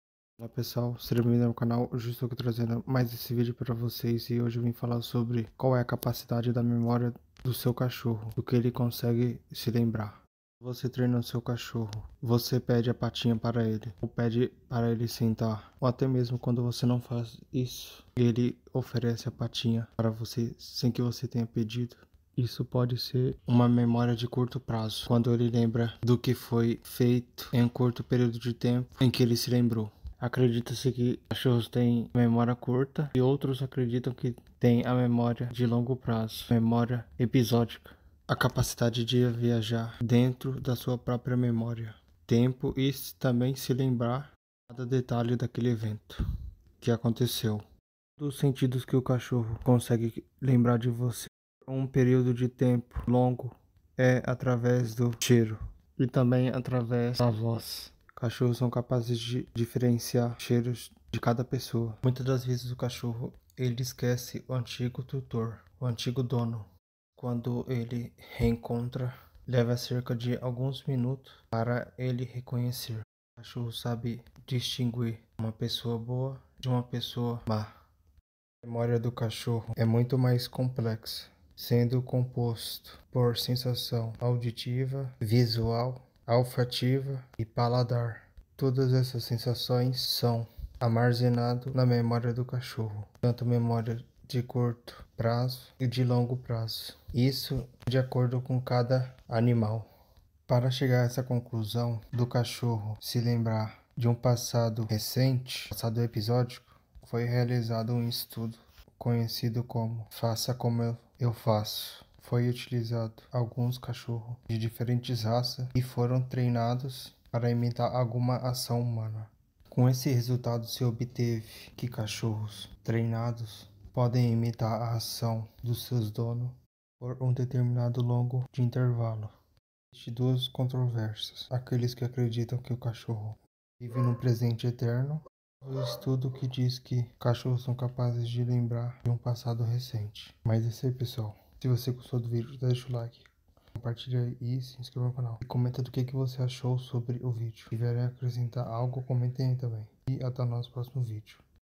QUAL é a CAPACIDADE da MEMÓRIA do seu CACHORRO expecpeixe dog - expecpeixe
@Expecpeixe  - Dia:9/1/2022- 12:50• janeiro

É Apenas conteúdo informativo e altamente qualificado em 4k

qual será a capacidade da inteligência de um cachorro

•obrigado a todos que assistiram